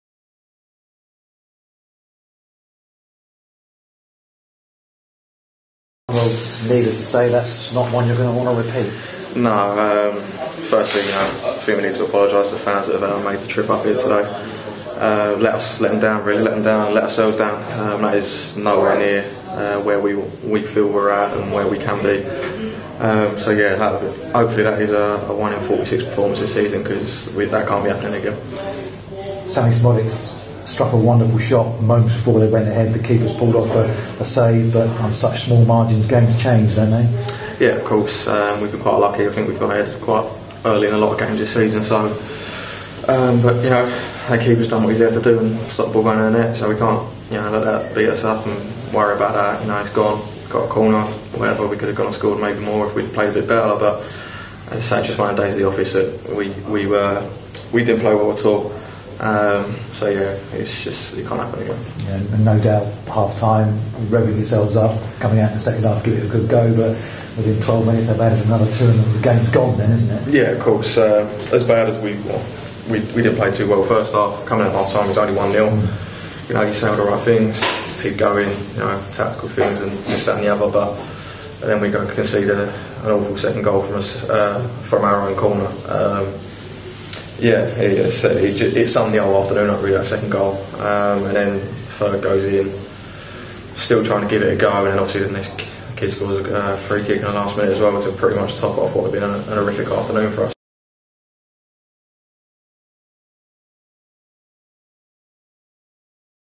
Lapslie Down After Defeat
Colchester United lost 4-0 at Carlisle United at the weekend, and midfielder Tom Lapslie was especially disappointed with the second half performance from the visiting U's.